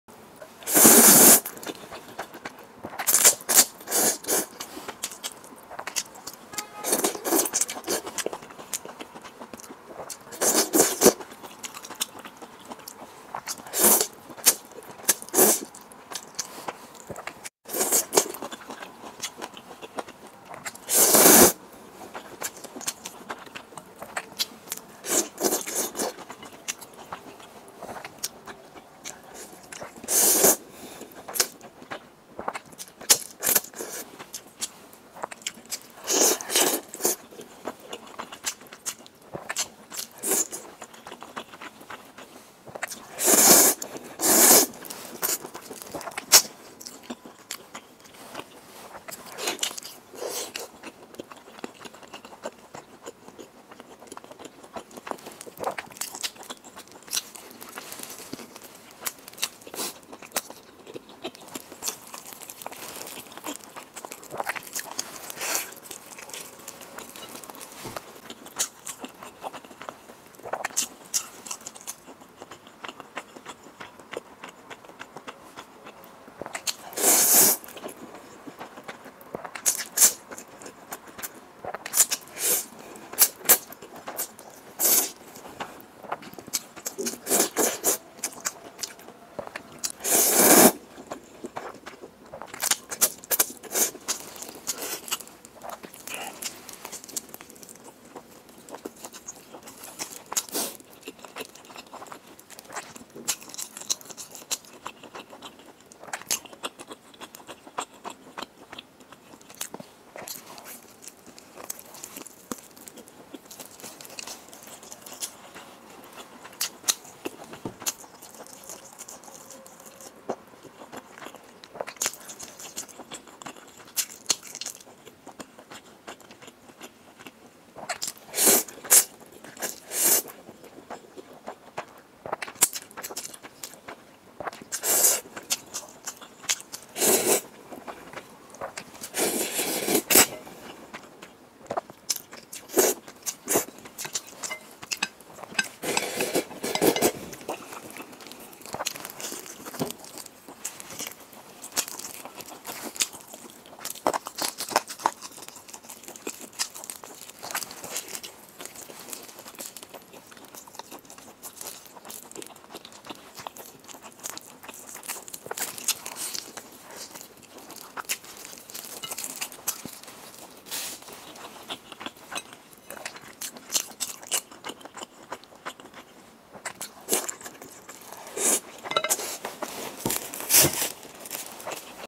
MUKBANG ASMR | 宫多捞面搭配大鸡腿🍗！QQ弹弹鲜鲜嫩嫩，好吃又满足！ 【深夜吃播花花】
MUKBANG ASMR | 宫多捞面搭配大鸡腿🍗！QQ弹弹鲜鲜嫩嫩，好吃又满足！

哈喽哈喽，这里是吃饭很香很香的花花🌸
如果你也喜欢和我一起吃饭的话